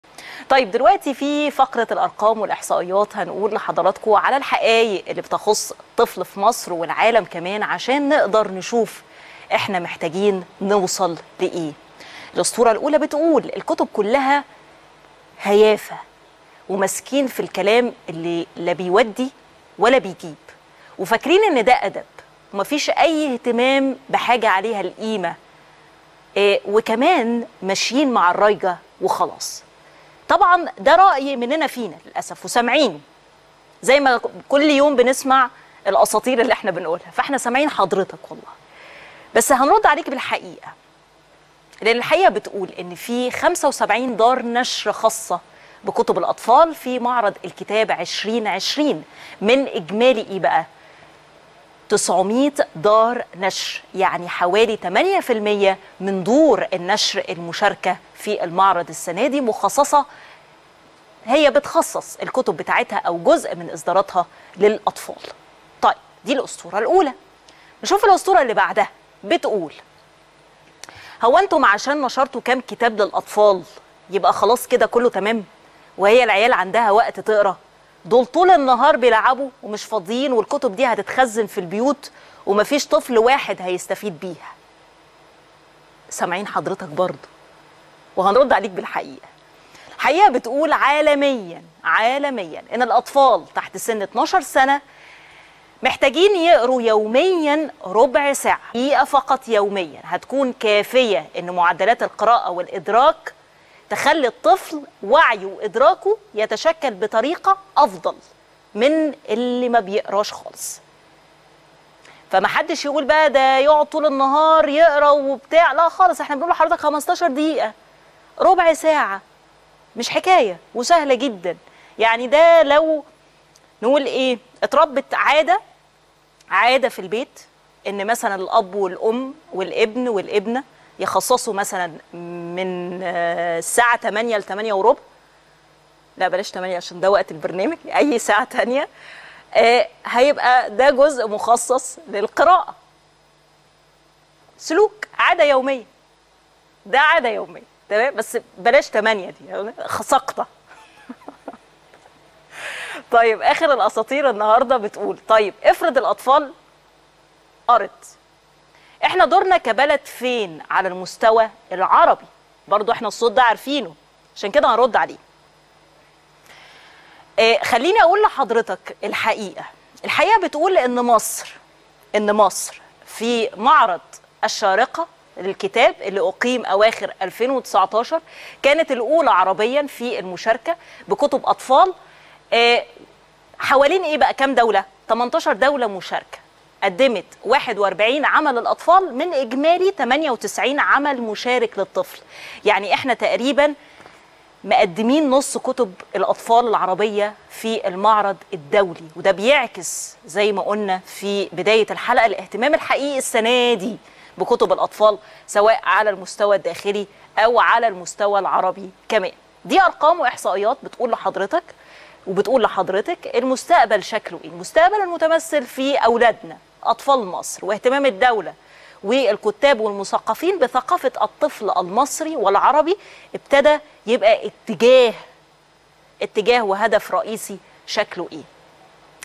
الحقيقة | 70 دار نشر تطرح إصدارات موجهة للأطفال.. وربع ساعة قراءة تكفي لتنمية إدراكهم
الحقيقة | معرض الكتاب يشهد ارتفاع في عدد الأنشطة الثقافية الموجهة للطفل